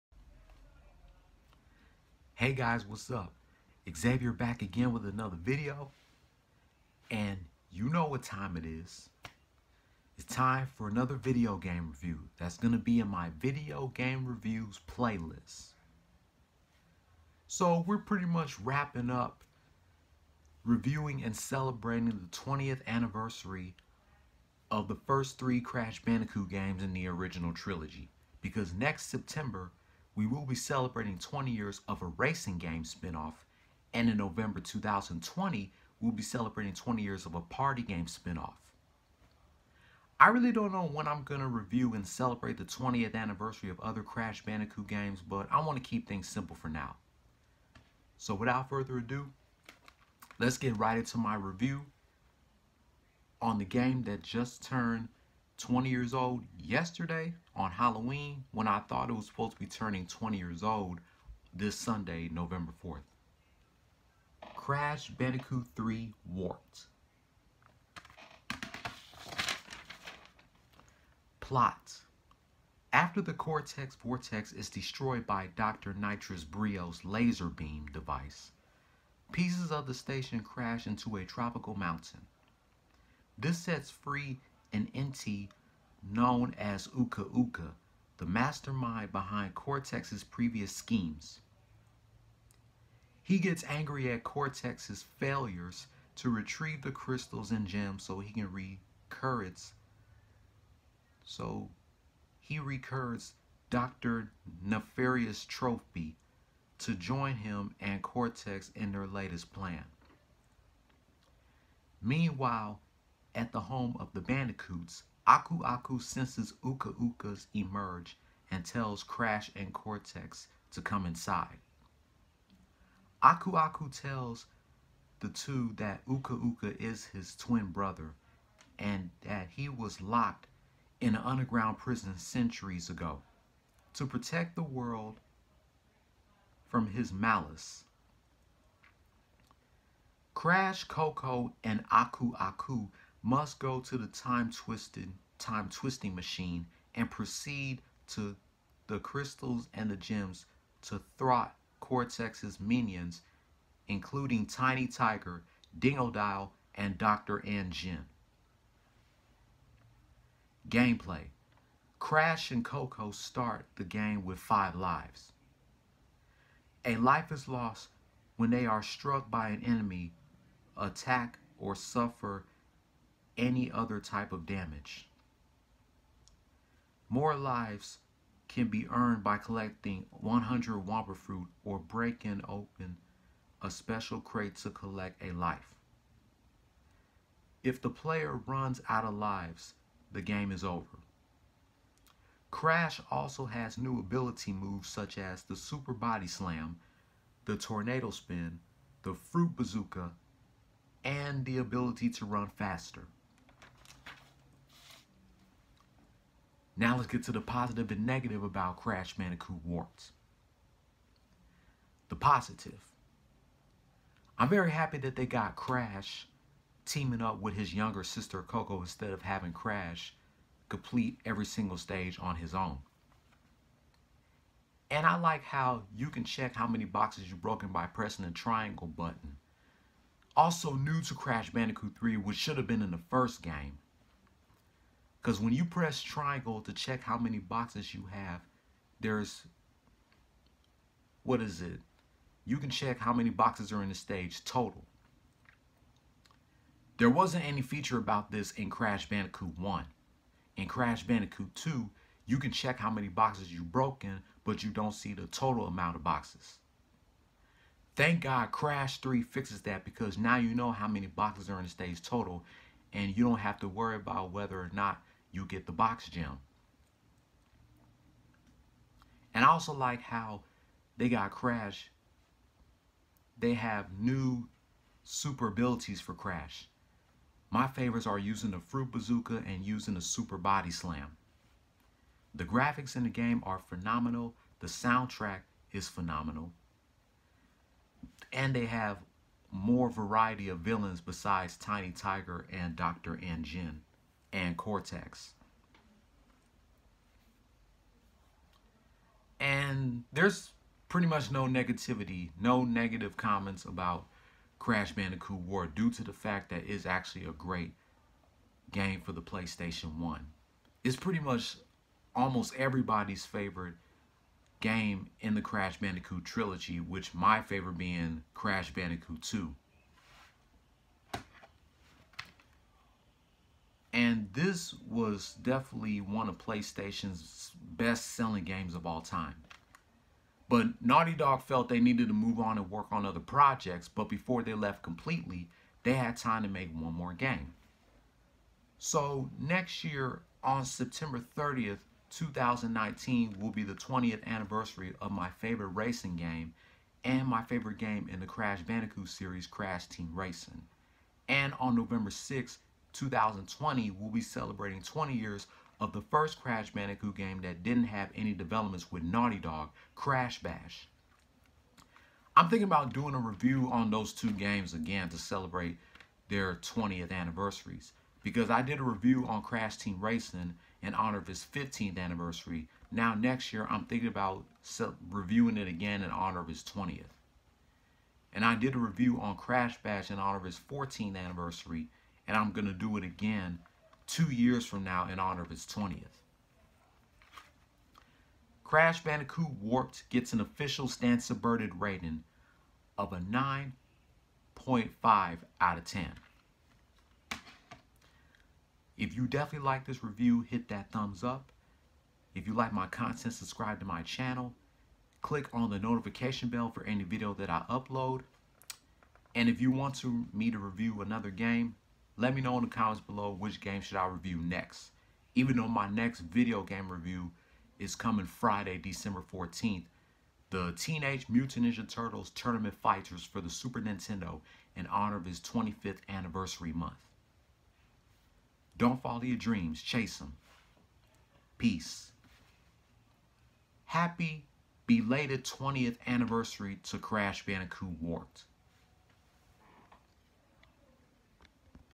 Crash Bandicoot Warped review
Here’s my review on Crash Bandicoot Warped in honor of its 20th anniversary